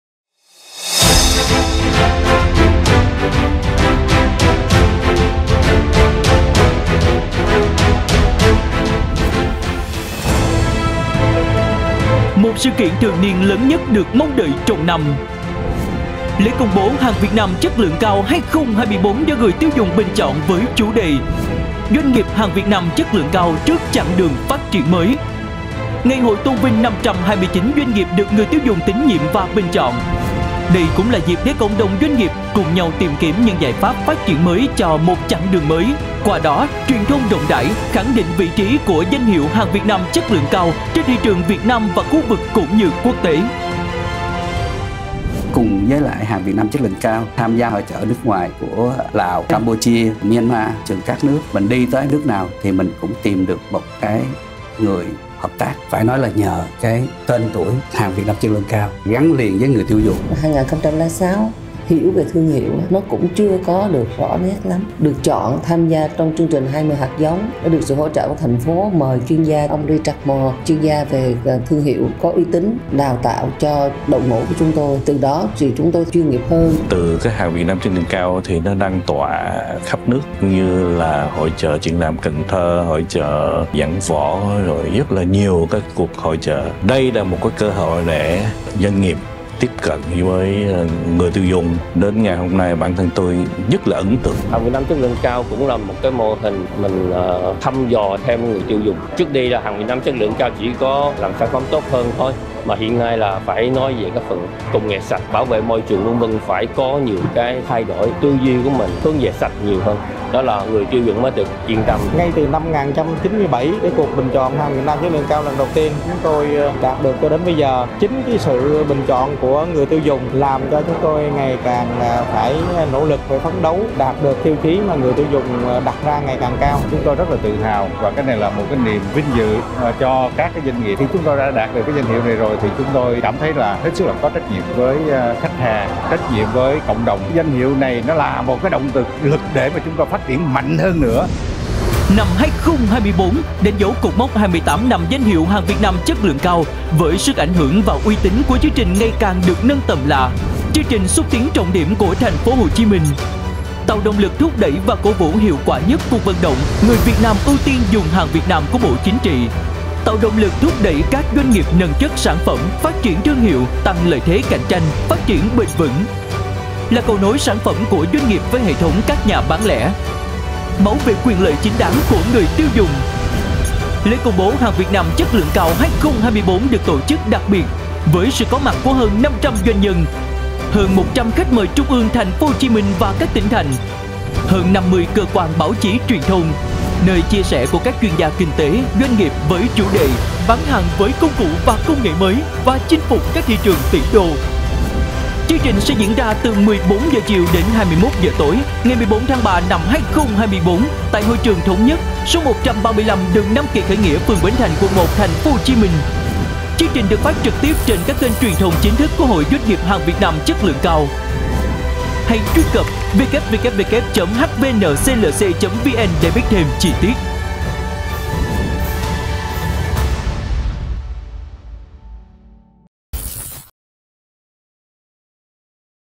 Trailer Lễ công bố hàng Việt Nam chất lượng cao 2024 - Doanh nghiệp trước chặng đường phát triển mới
Lễ công bố HVNCLC 2024, diễn ra tại Hội trường Thống Nhất, số 135 đường Nam Kỳ Khởi Nghĩa, P.Bến Thành, Q.1, TP.HCM vào ngày 14/3 tới đây.

Lễ công bố HVNCLC 2024 do người tiêu dùng bình chọn, có chủ đề: “Doanh nghiệp HVNCLC trước chặng đường phát triển mới”. Sự kiện có mặt hơn 500 doanh nhân, hơn 100 khách mời, hơn 50 cơ quan báo chí - truyền thông. Chương trình được phát trực tiếp trên các kênh truyền thông xã hội của Hội DN HVNCLC.

Lễ công bố là sự kiện đặc biệt nhất hàng năm của cộng đồng DN HVNCLC. Là nơi vinh danh 529 doanh nghiệp có sản phẩm chất lượng được người tiêu dùng bình chọn. Cũng là nơi tổ chức ngày hội HVNCLC 2024, với sự tham dự, gặp gỡ của các tổ chức xúc tiến thương mại, nhà mua hàng trong nước và quốc tế.